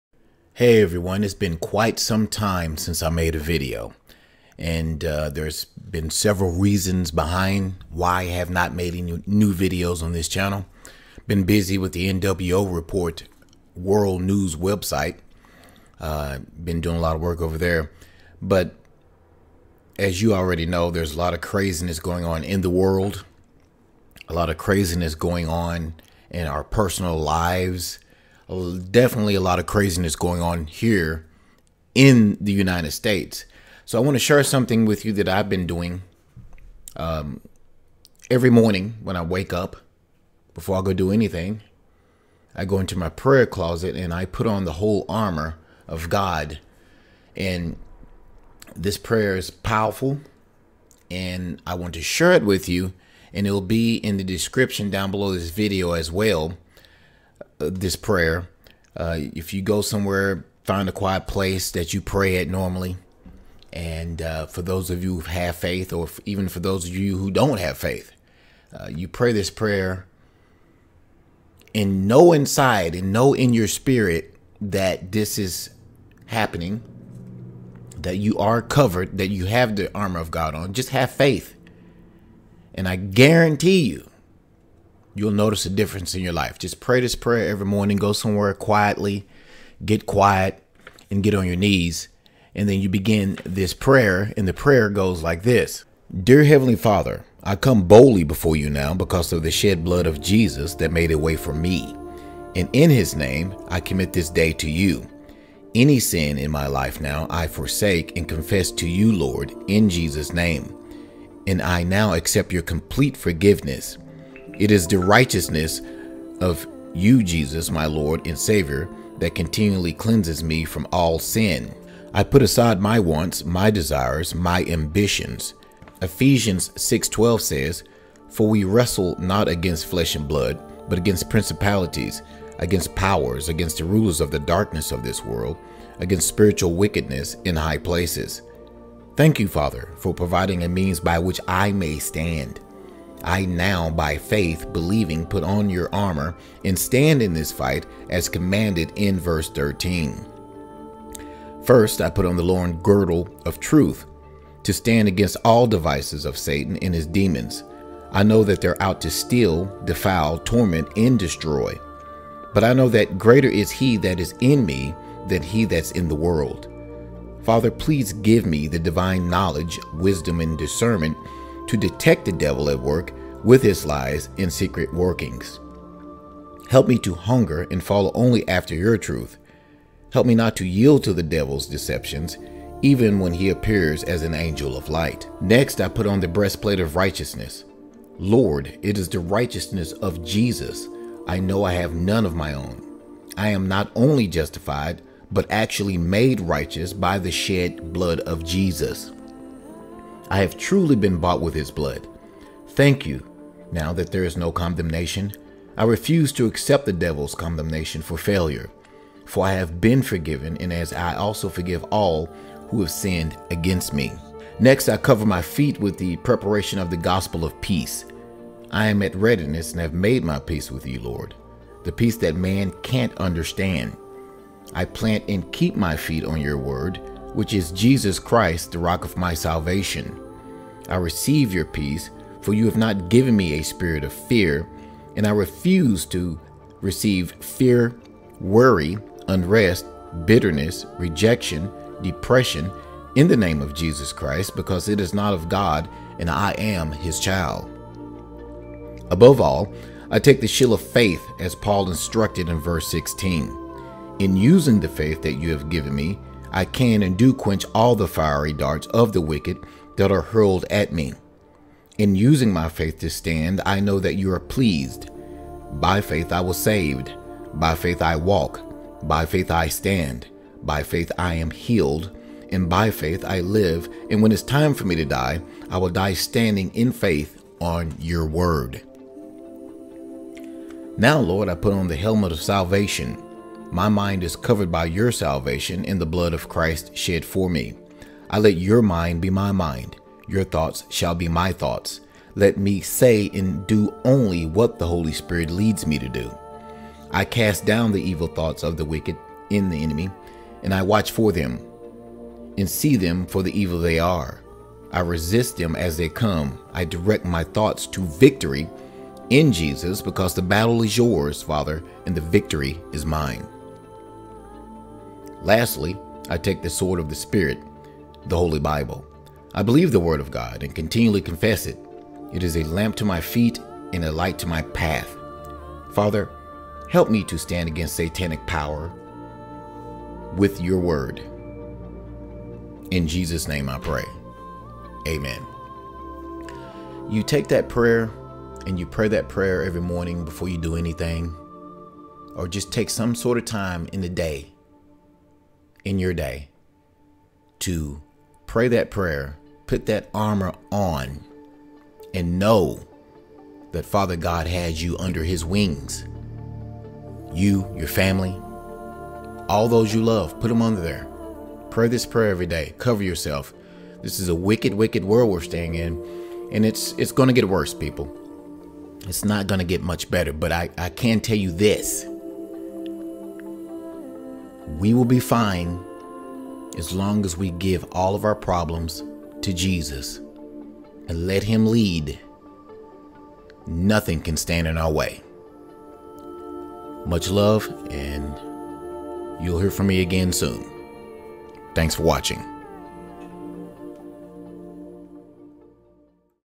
PUT ON THE WHOLE ARMOR OF GOD!
With all the crazy things going on right now, we need protection from the darkness in this world. This is how you get protected!

EPHESIANS 6:11-17 within these words rest the basics for the Christian life, the Christian walk, and the Christian stand during troubles and trials. Sometimes we think the basics are “too basic” to be important. But until we learn and master the basics-the “advanced” never fits right. And we will always have to use these fundamentals because they are “basic” to our spiritual battle. Everything physical is related to the spirit world.
Take time-get alone (if you can)-quiet before the Lord-and read this to yourself. Think about the meaning (and power)-then read it aloud as a prayer to God and notice to the spiritual world as to your intentions and your stand.

“Dear heavenly father, I come boldly before you now because of the shed blood of Jesus that made a way for me, and in his name, I commit this day to you. Any sin in my life I now forsake and confess to you Lord, in Jesus’ name. And I now accept your complete forgiveness. It is the righteousness of you, Jesus, my Lord, and Savior that continually cleanses me from all sin. I put aside my wants, my desires, and my ambitions.  “
Ephesians 6:12 says, “For we wrestle not against flesh and blood, but against principalities, against powers, against rulers of the darkness of this world, against spiritual wickedness in high places.”
Thank you, Father, for providing a means by which I may stand. I now by faith-believing-put on your armor and stand in this fight as commanded in verse 13.

    LOIN GIRDDLE OF TRUTH

“First, I put on the Loin Girddle of truth to stand against all devices of Satan and his demons. I know they are out to steal, defile, torment, and destroy. But I know that “greater is he that is in me than he who is in the world.” Father, please give me the divine knowledge, wisdom, and discernment to detect the devil at work with his lies and secret workings. Help me hunger and follow only after your truth. Help me not yield to the devil’s deceptions even when he appears as an angel of light.”

BREASTPLATE OF RIGHTEOUSNESS

“Next, I put on the breastplate of the righteousness of Jesus. I know I have none of my own. I am not only “justified” but also actually made “righteous” by the shed blood of Jesus. I have truly been bought with his blood. That you know that there is no condemnation. I refuse to accept the devil’s condemnation for failure, for I have been forgiven even as I also forgive all who have sinned against me.”

SHOES OF PEACE

“I cover my feet with the preparation of the Gospel of Peace. I am in readiness and have made my peace with you, Lord. The peace that man cannot understand. I plant and keep my feet on your word, which is Jesus Christ, the rock of my salvation. I receive your peace, for you have not given me a “Spirit of fear,” and I refuse to receive fear-worry-unrest-bitterness-rejection-depression in the name of Jesus Christ because it is not of God and I am his child.

SHIELD OF FAITH

“Above all, I take the Shield of faith as Paul instructed in verse 16. In using the faith that you have given me, I can and do quench all of the fiery darts of the wicked that are hurled at me. In using my faith to stand, I know that you are pleased. By faith, I was saved. By faith, I walk. By faith, I stand. By faith, I am healed. By faith, I live and when it is time for me to die-I will die standing in faith on your word.

HELMET OF SALVATION

“Now Lord, I put on the Helmet of Salvation. My mind is covered by your salvation and the blood of Christ shed for me. I let your mind be my mind. Your thoughts shall be my thoughts. Let me say and do only what the Holy Spirit leads. I cast down the evil thoughts of the enemy. I watch for them and see them for the evil they are. I resist them as they come. I direct my thoughts to victory in Jesus because the battle is yours, Father, and the victory is mine!”

      SWORD OF THE SPIRIT

“Lastly, I take the Sword of the spirit –the Holy Bible. I believe the Word of God, and continually confess it. It is a lamp to my feet and a lamp to my path. Father, help me stand against satanic powers with Your Word.”

CONCLUSION

Dear Christian Friend: Realize who you are in Jesus. The Bible says you are an overcomer and a child of God. Therefore, begin each day by realizing you are who the Bible says you are. Pray this prayer faithfully each day until it because as much a part of your life as brushing your teeth. You HAVE authority and power over the enemy. God wants you to trust him. He desires for signs and wonders to be the norm; sick people healed, the lost saved, and the bound set free. So read his Word faithfully, putting on God’s armor. Pray, praise, sing, and believe God’s Word as never before.